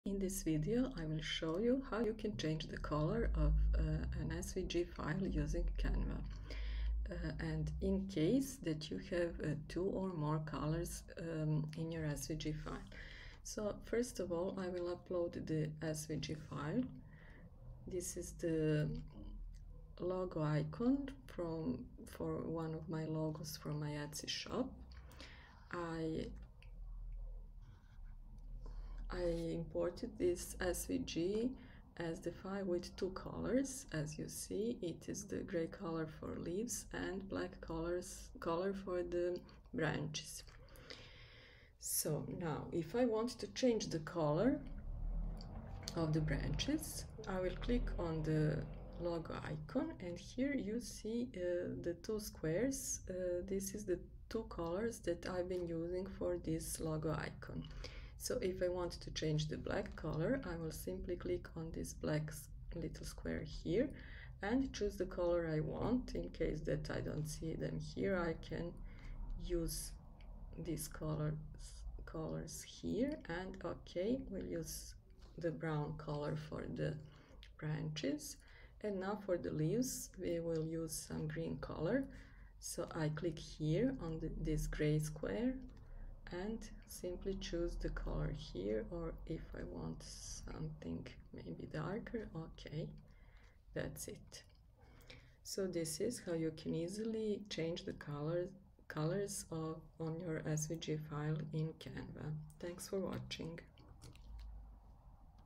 How to change the color of an SVG file using Canva - two or more colors
A short video tutorial - how to change the colors on your SVG file using Canva